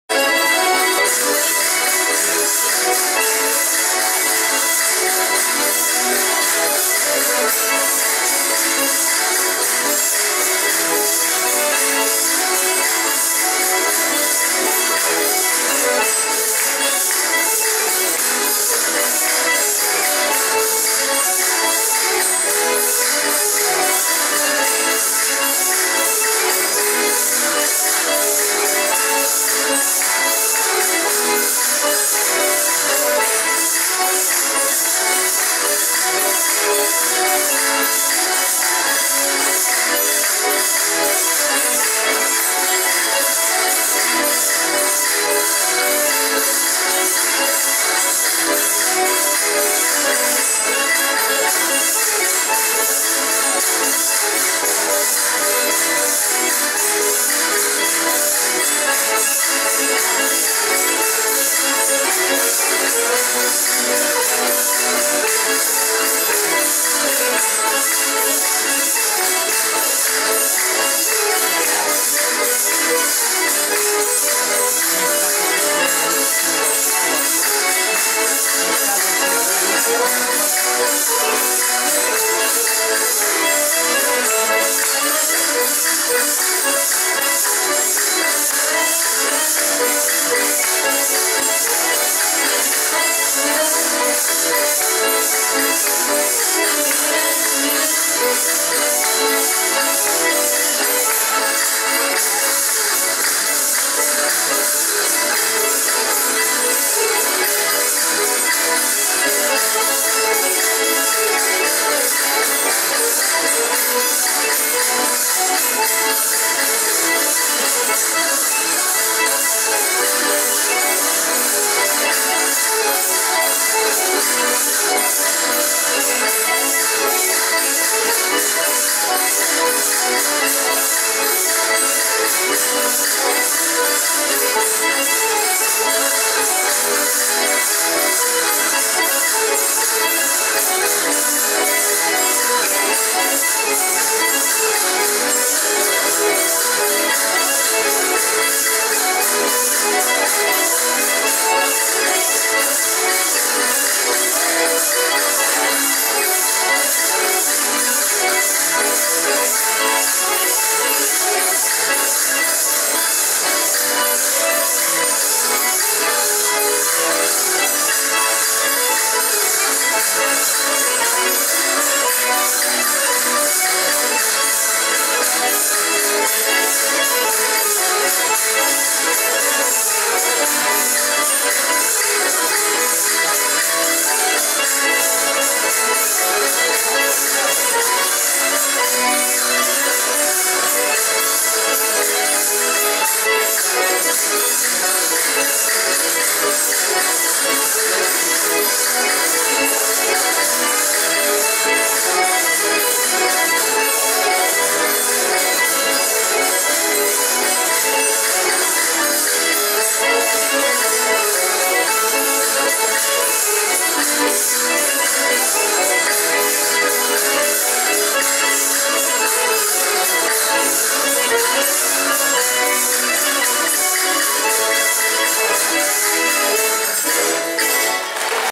II GALA INTERNACIONAL ACORDEÃO ESTOI 2013
Grupo ACESTOI, actuando na II Gala Internacional Acordeão Estoi 2013, Faro, Portugal